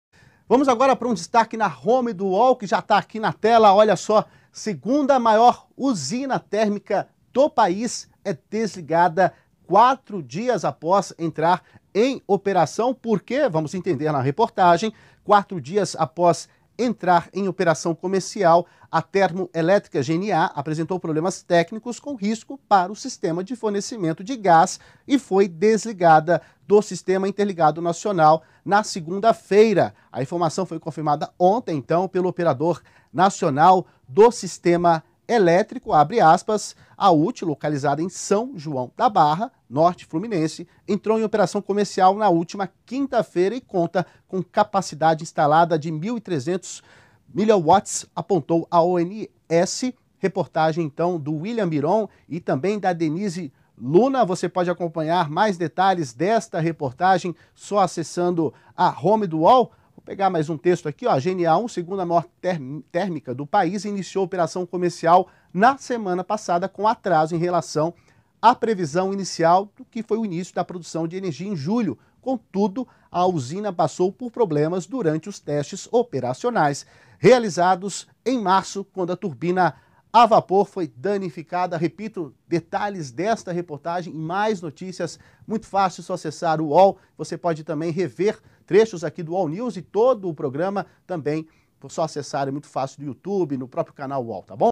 Segunda maior usina térmica do país é desligada 4 dias após entrar em operação
Quatro dias após entrar em operação comercial, a termoelétrica GNA I apresentou problemas técnicos com risco para o sistema de fornecimento de gás e foi desligada do Sistema Interligado Nacional (SIN) na segunda-feira, 20, às 11h38. A informação foi confirmada ontem pelo Operador Nacional do Sistema Elétrico (ONS)